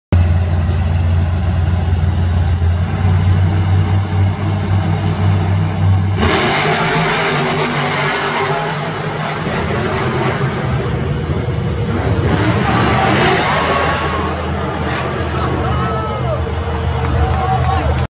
Monster Jam Melbourne 2019 Show 1 Round 1 El Toro Loco vs Megalodon
I own this footage!